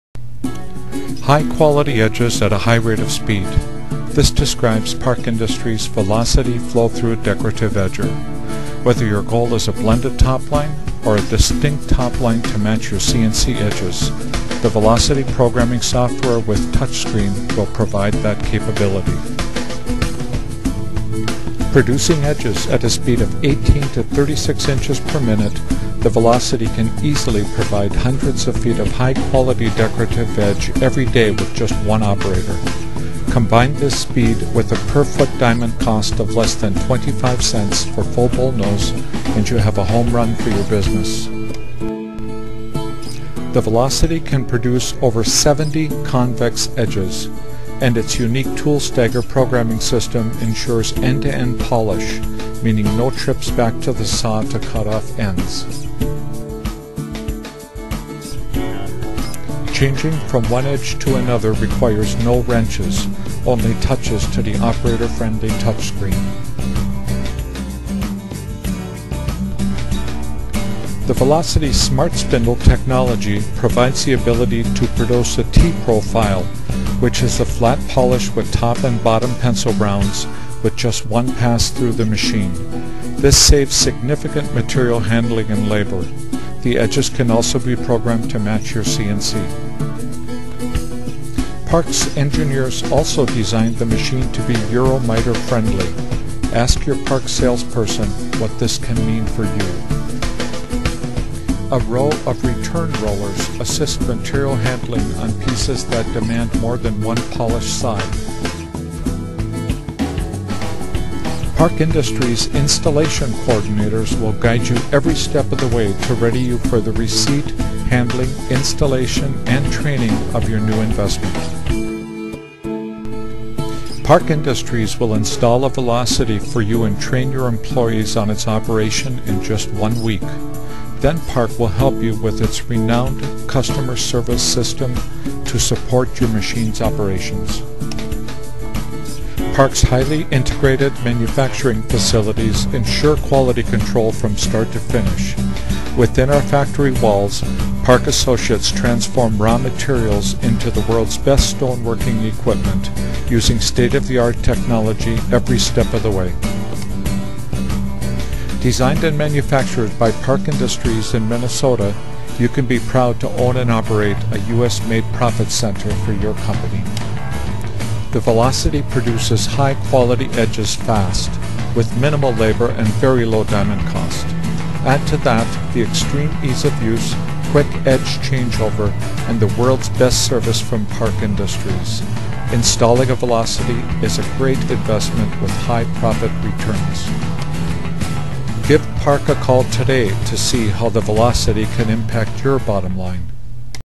Velocity Straight Line Polisher - Park Industries
DECORATIVE EDGE SHAPING AND POLISHING
FOR SPEED AND PRECISION

Maximize productivity with the VELOCITY -- Park's decorative edger that offers a perfect edge finish in a single pass. Operators can easily shape and polish laminate edges in a single pass without the hassles of double-cutting on a saw.